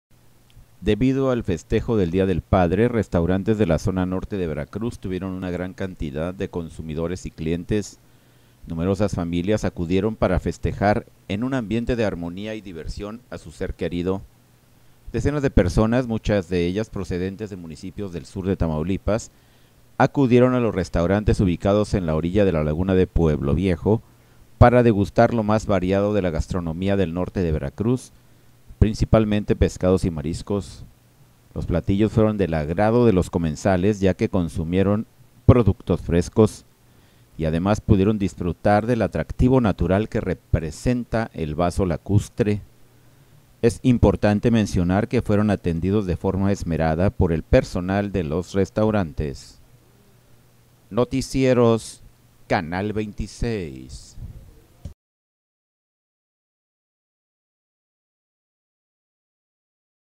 LLENOS POR DÍA DEL PADRE RESTAURANTES DE PUEBLO VIEJO.
Debido a la festividad del día del padre, restaurantes de la zona norte de Veracruz tuvieron una gran cantidad de consumidores y clientes.